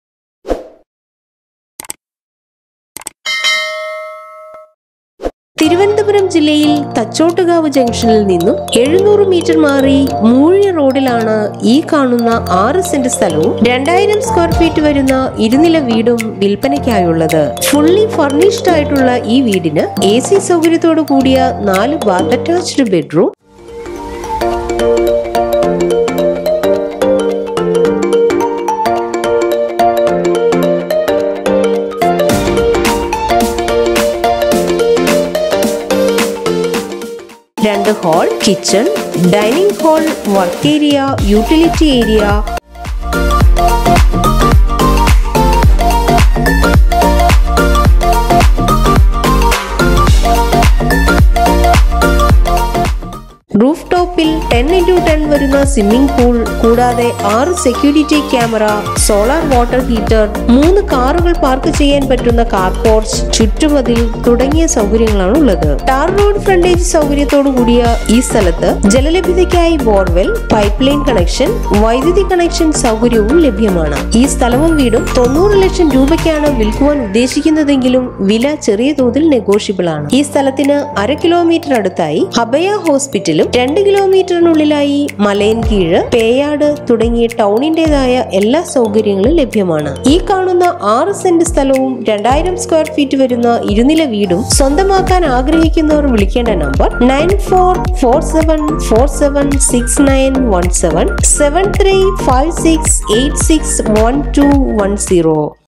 HOUSE FOR SALE IN TRIVANDRUM|FULLY FURNISHED HOUSE IN THACHOTTUKAVU| chandrasekhar tvm 01 0924 varun
Trivandrum district 700 m from Thachottukavu jn , Moozhi road 6 cent land and 2000 sqft fully furnished two storied house. house consist 4 bath attached a/c bedrooms, 2 hall, kitchen, dining hall, work area, utility area, 10*10 swimming pool in roof top, 6 security camera, solar water heater, inverter and car porch. 4 side compound wall.tar road fronatge. bore well, pipe line connection for water facility.
within 500 m Abhaya hospital 2 km to Malayankeezhu and Peyad